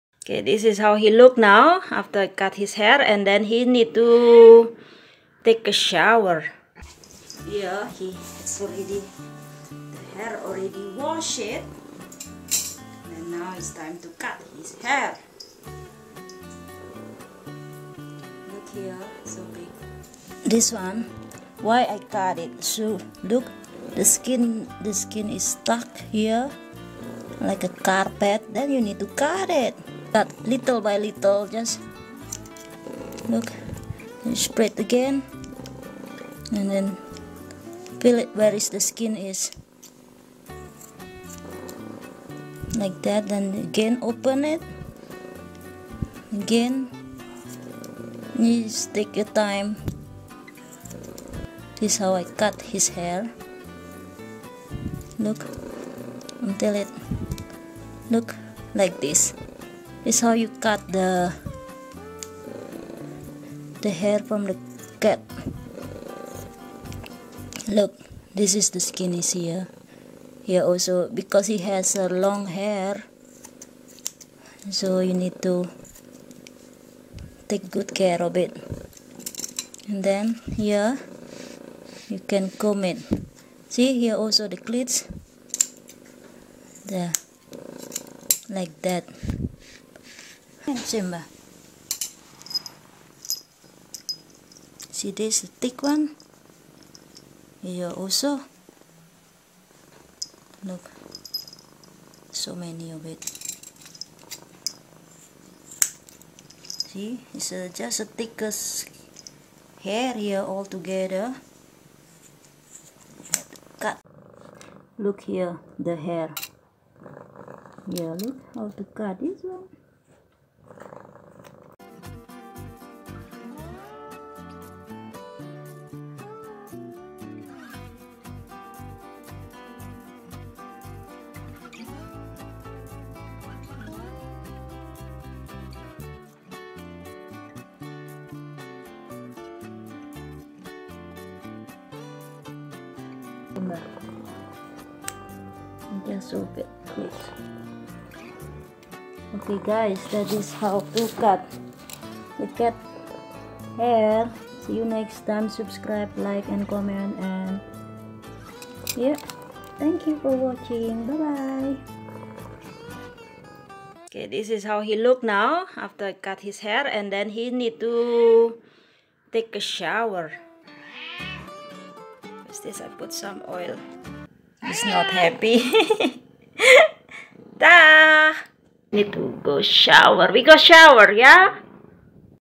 How I Cut my Cat Hair at Home || Haircut Cat Tutorial
Haircut for the cat is easy you just need to careful with the skin and take extra time to get it done